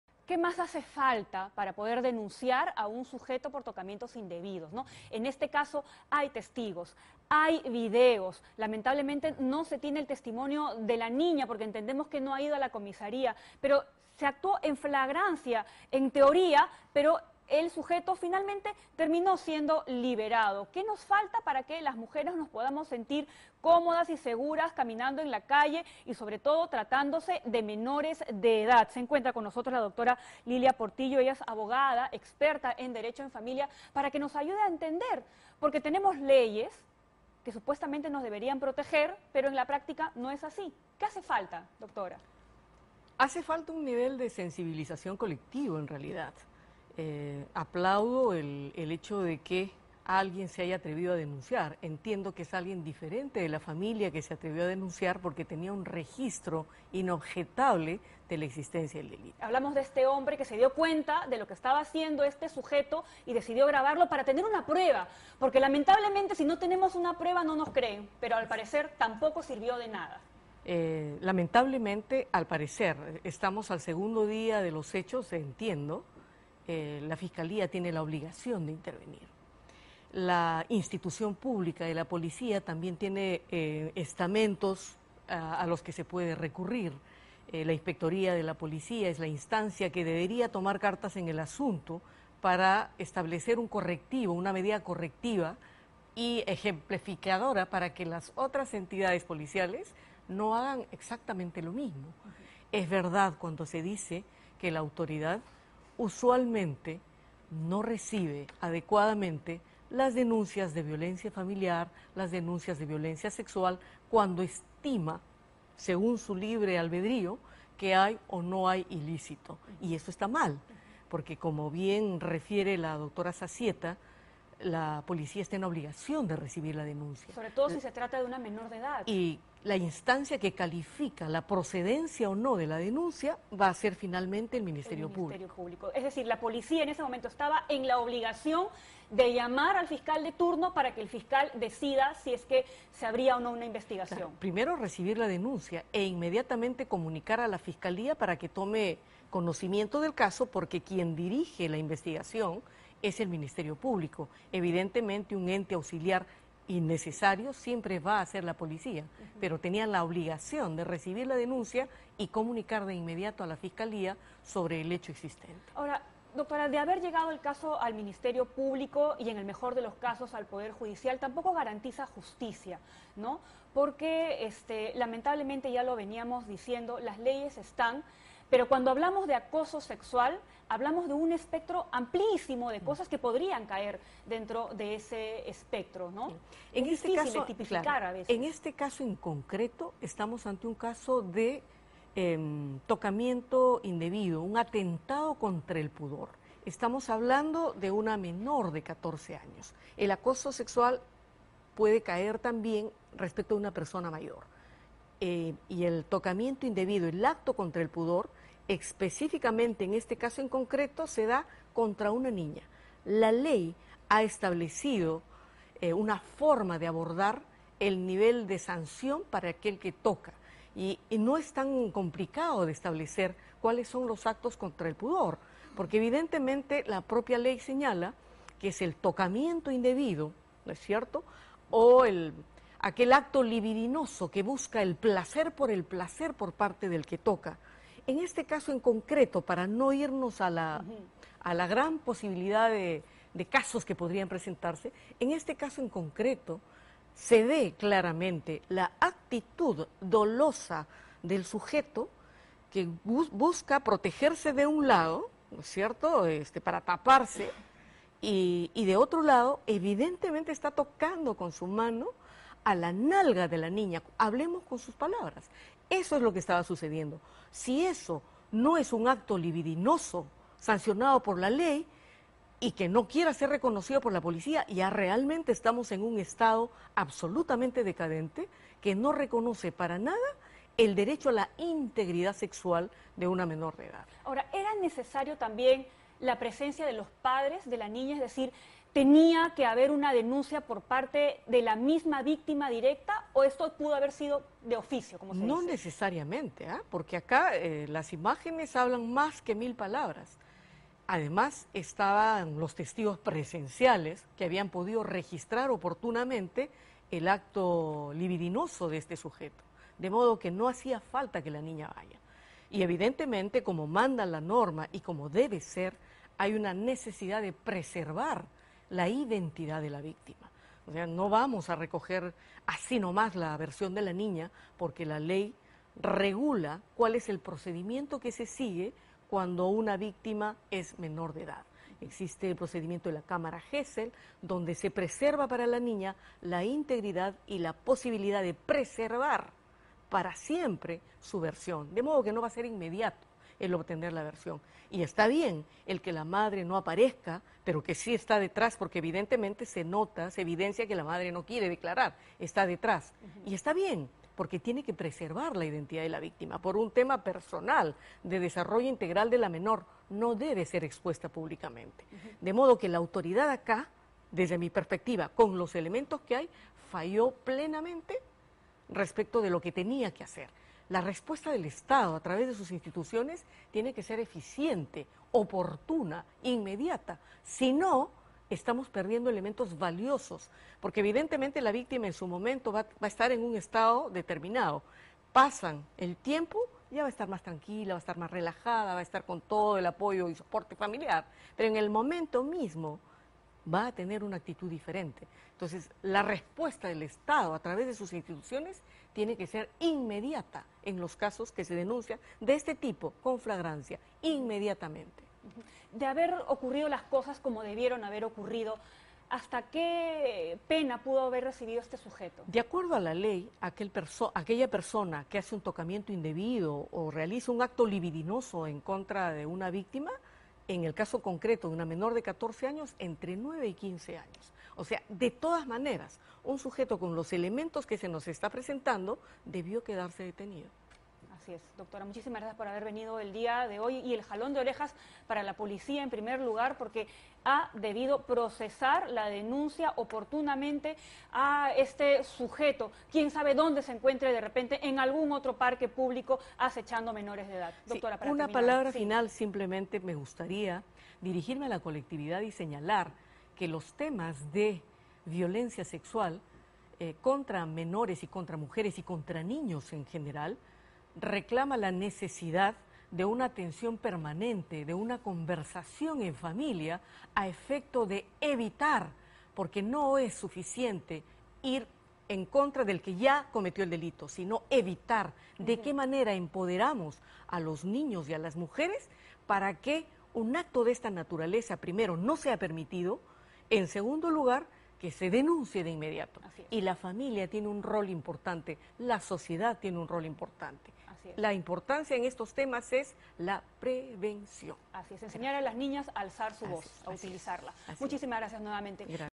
¿Cuáles son las penas por tocamientos indebidos a niños?, experta opina
Indignante. Un hombre terminó realizando tocamientos indebidos a una menor que se encontraba disfrutando de una espectáculo a otras pequeñas en un parque público en Jesús María. Este acto fue grabado por un testigo pero pese a la evidencia el hombre fue liberado. Pero ¿Qué hace falta para poder denunciar a personas que realizan este tipo de actos.?